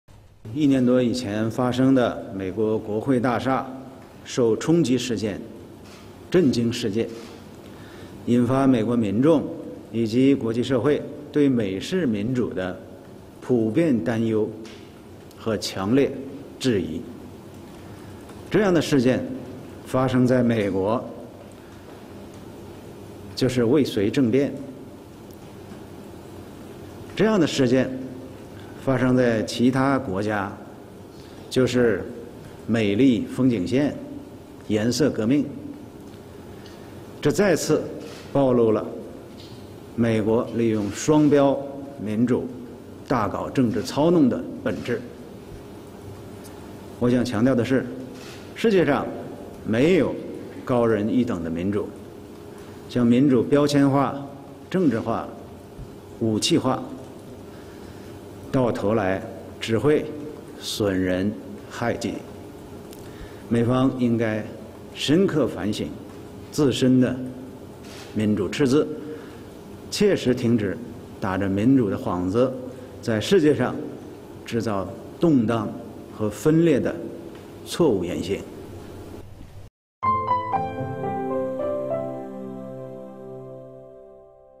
赵立坚谈美方宣布国会山骚乱为“未遂政变” ：美应深刻反省自身“民主赤字”
中国外交部发言人赵立坚在6月10日的例行记者会上表示，一年多以前发生的美国国会大厦受冲击事件震惊世界，引发美国民众以及国际社会对美式民主的普遍担忧和强烈质疑。这样的事件发生在美国就是“未遂政变”，发生在其他国家就是“美丽风景线”“颜色革命”，这再次暴露了美方利用“双标”民主大搞政治操弄的本质。
        赵立坚强调，世界上没有高人一等的民主，将民主标签化、政治化、武器化，到头来只会损人害己。美方应当深刻反省自身的“民主赤字”，切实停止打着民主幌子在世界上制造动荡和分裂的错误言行。